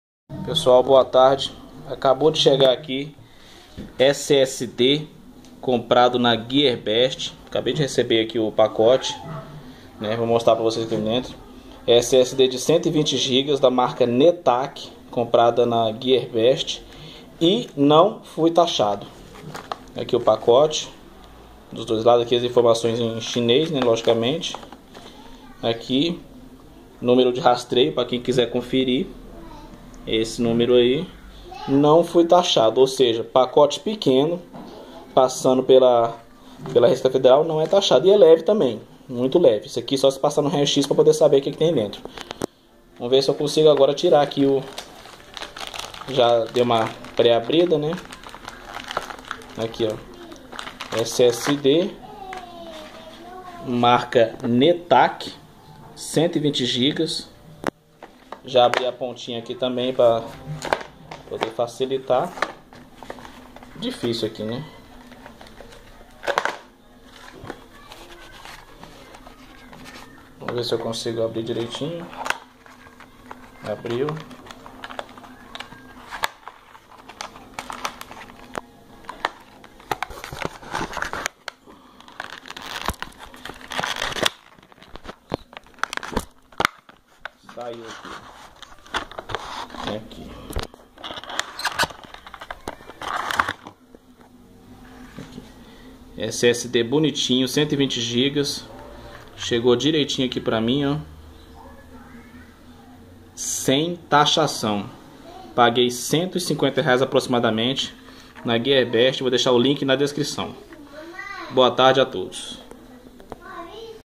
SSD 120gb comprado na Geabest (NÃO FUI TAXADO)
SSD de 120GB da marca Netac comprado na Gearbest, paguei aproximadamente R$ 150,00. Não fui taxado pela receita federal. Instalei o ssd no meu notebook e está funcionando perfeitamente. Ao comprar enviei ao vendedor a mensagem " Send as Dropshipping" para que o mesmo pudesse me ajudar a passar pela receita federal sem problemas de taxação. Valeu muito a pena. Código de rastreio dos correios: RF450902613SG.